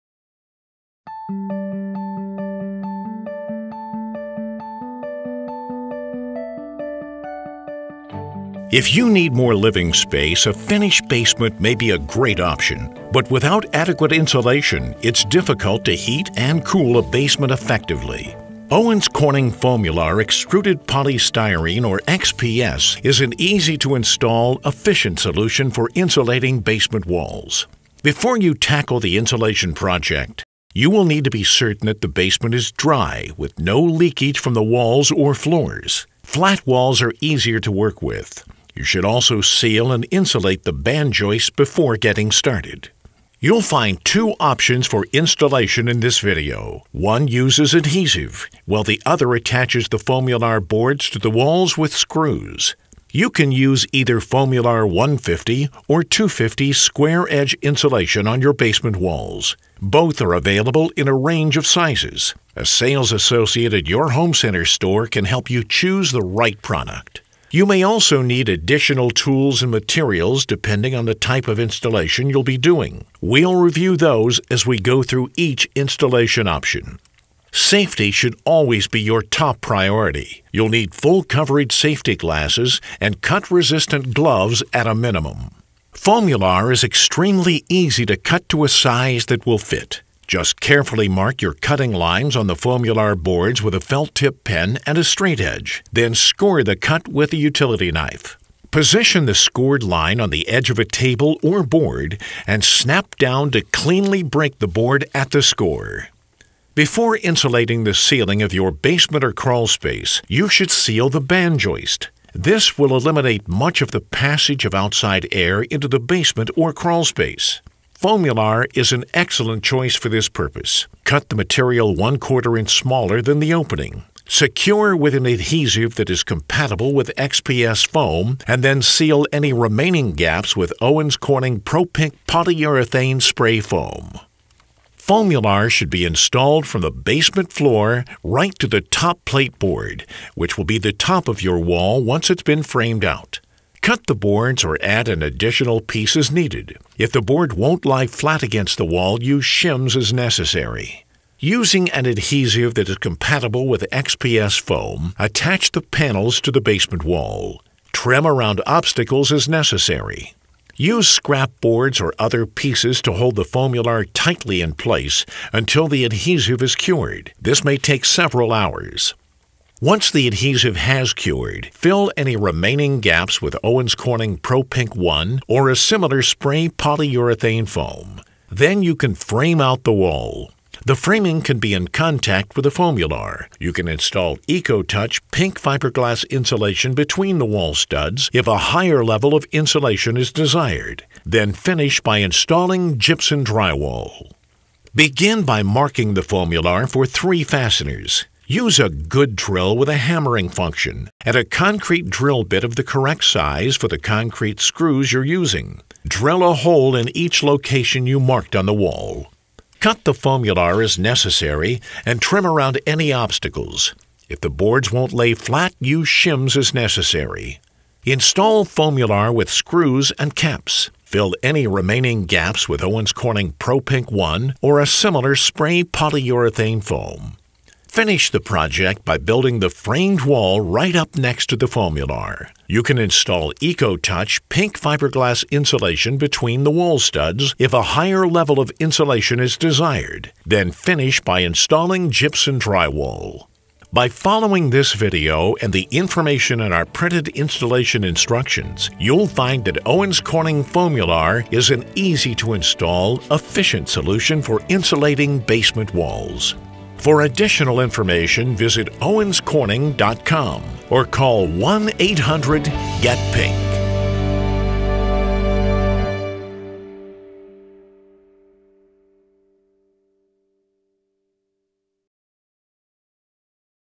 Insulation Installation Guide: FOAMULAR® XPS Insulation for Basement Walls
If you need more living space, a finished basement is a great option. It can be difficult to maintain a comfortable temperature. Using Owens Corning FOAMULAR® XPS insulation for basement walls is an efficient solution. This video details two different methods for installing FOAMULAR® XPS insulation — one that uses adhesive and one that uses screws. It also reviews the tools, materials, and safety gear you will need for your basement insulation project.

To learn more about Owens Corning’s insulation products and solutions,